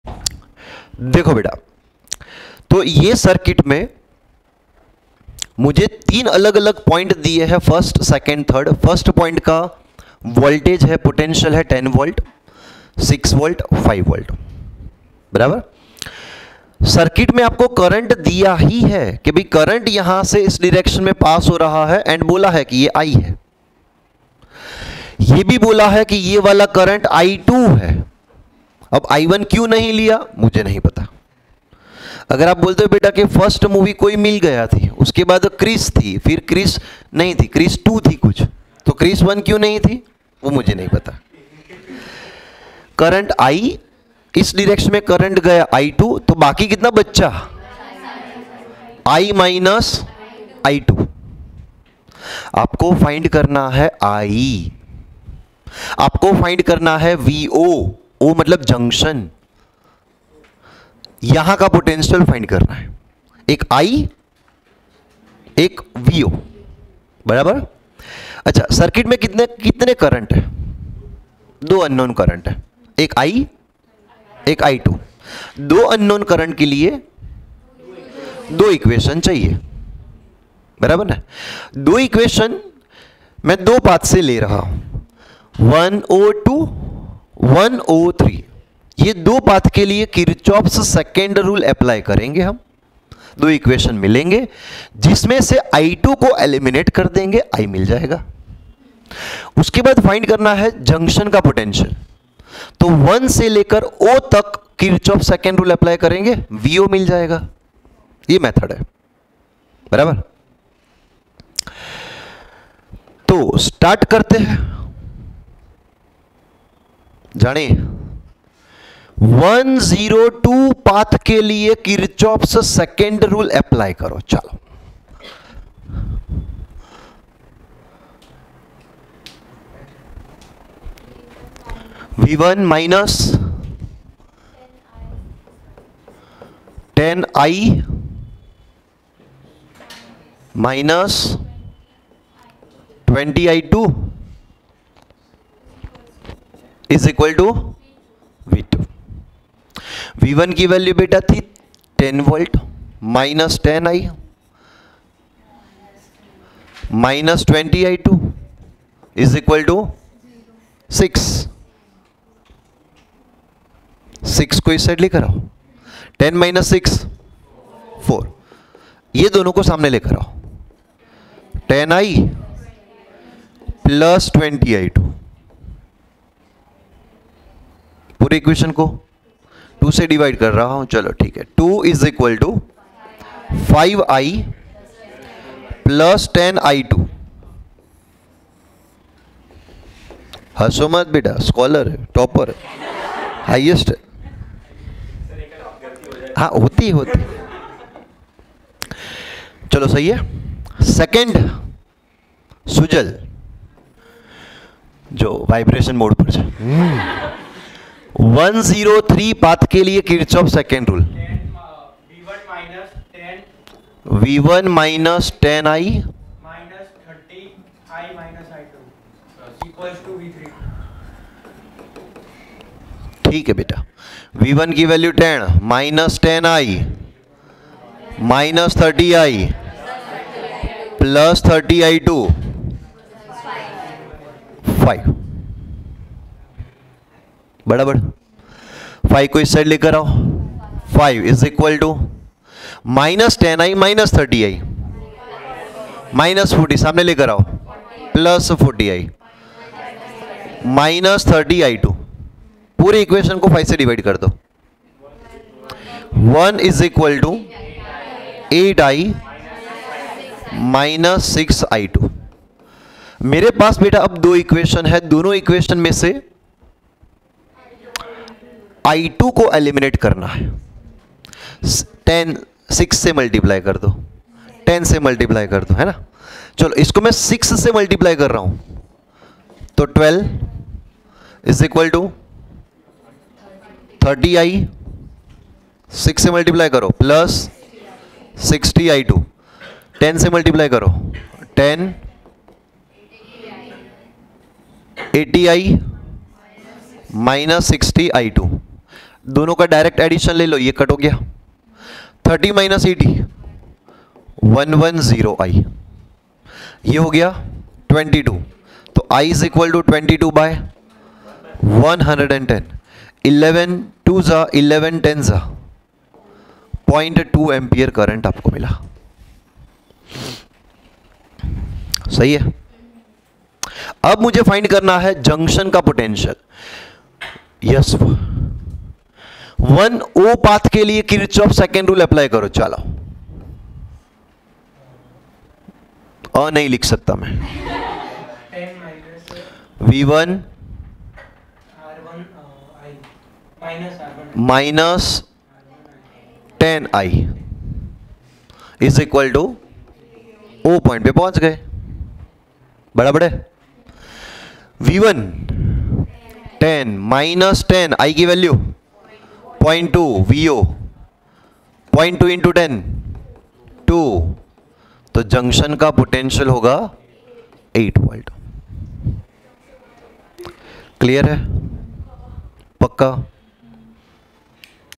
(16) Calculate the current flowing through the resistor R1 in the given circuit. R1 = 10 ,...
(16) Calculate the current flowing through the resistor R1 in the given circuit. R1 = 10 , R2 = 20  and R3 = 30. The potentials of the points 1,2 and 3 are respectively, V1 = 10V, V2 = 6V and V3 = 5 V. the potential at the junction is I = ….and V0 …..